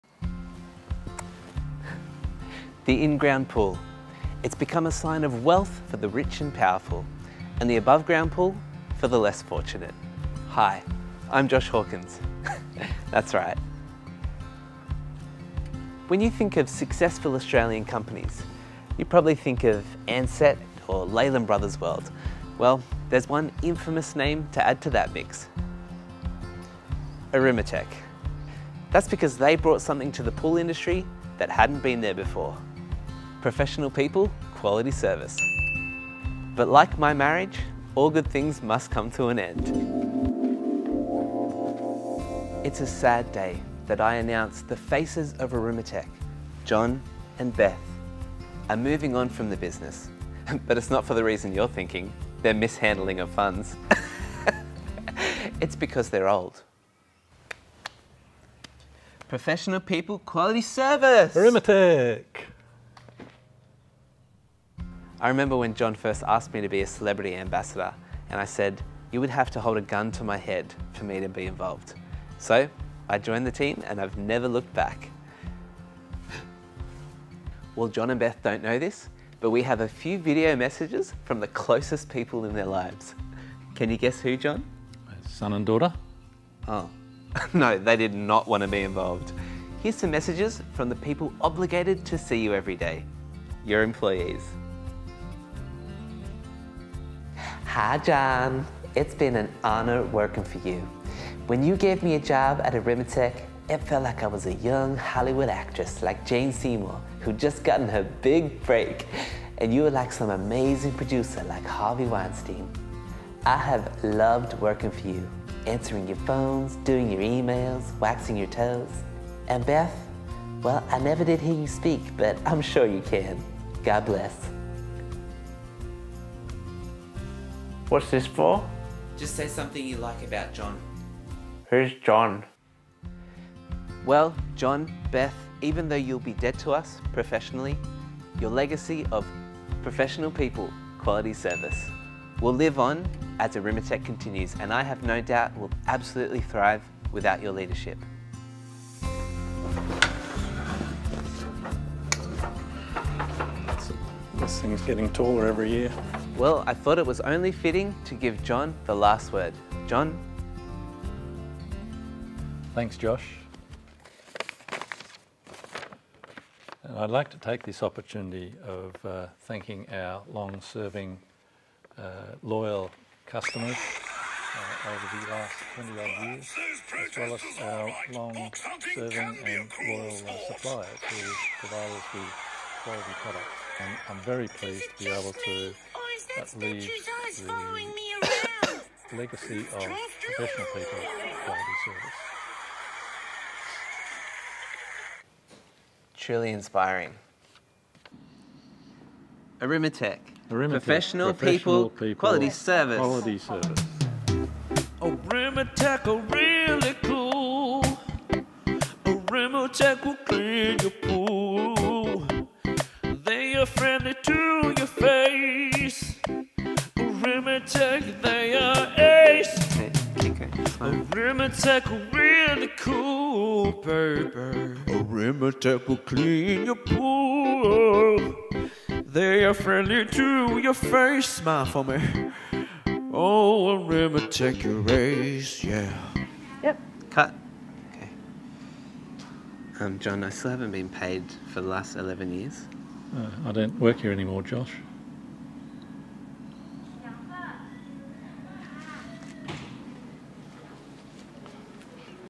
Orimatech: The Finale (Season 8)
It's all come to an end, not surprisingly.
Orimatech as we know it, will not continue. What a ride it's been John and Beth. Take care of yourselves as you retire and eventually pass from this life. Love you lots... everyone ❤️